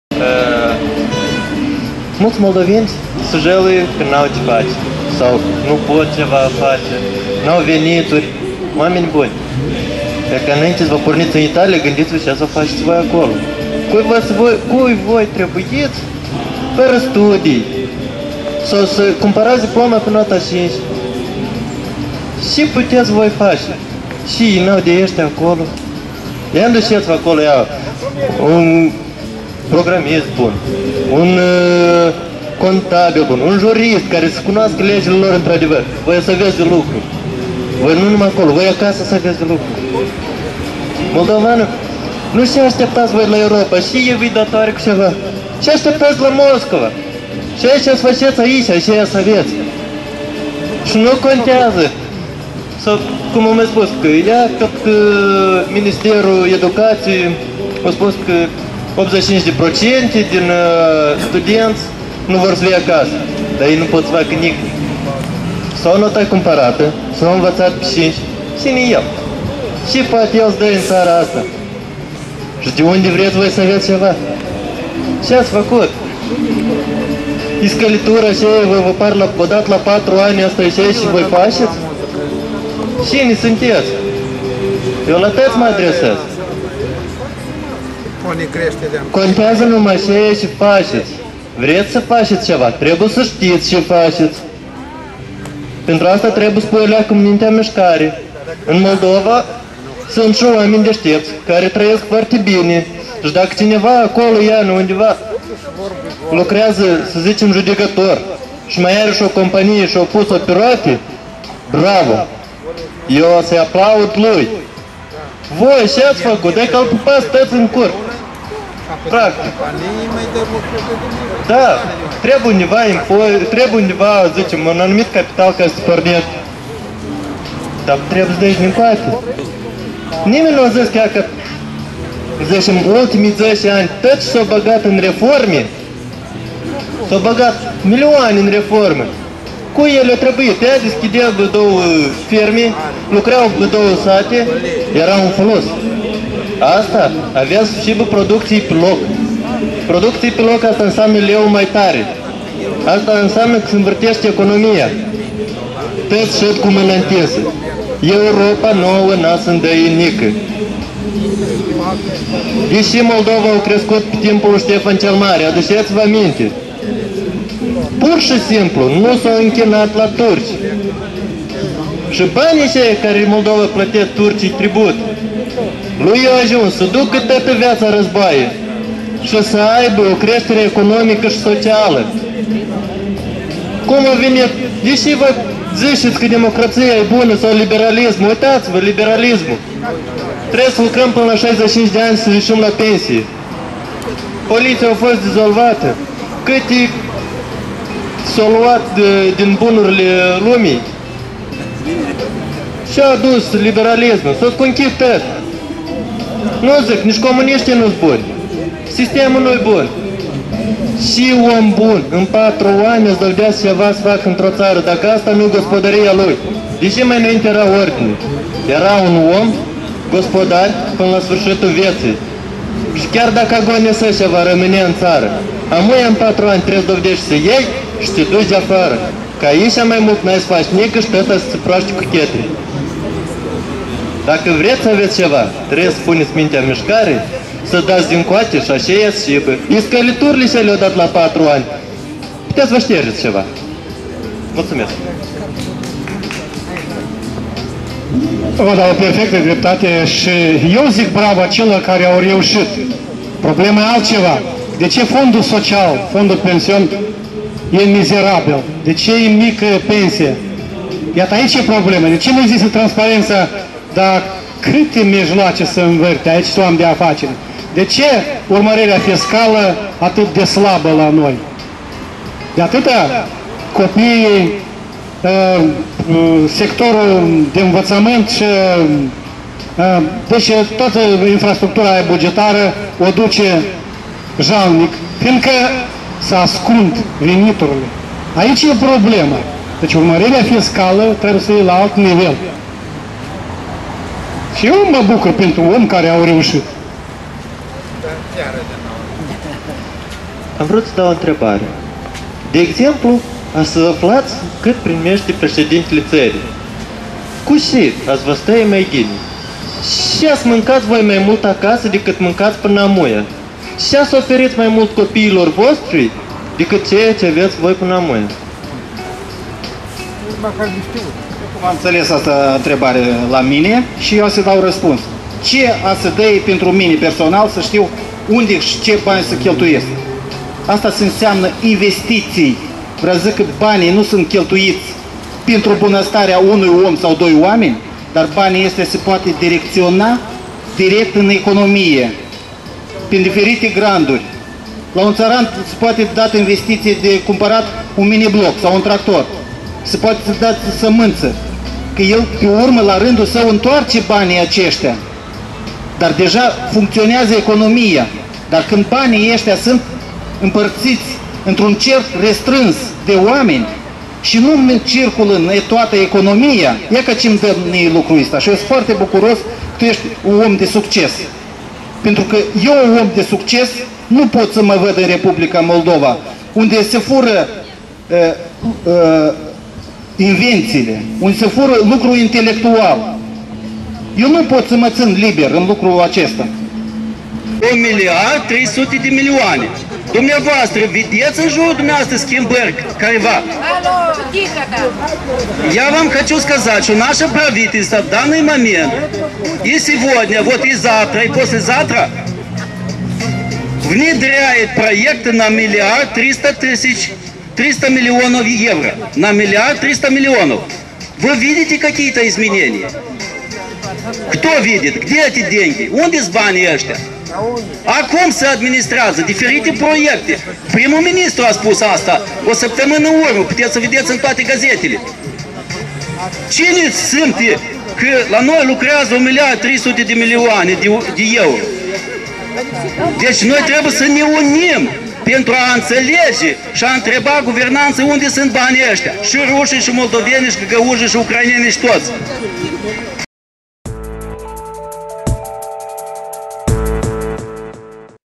Cui trebuim noi în Europa, dezbateri în park
Discuţii la Microfonul liber, proiect duminical al asociaţiei Hyde Park, Chişinău, aleea clasicilor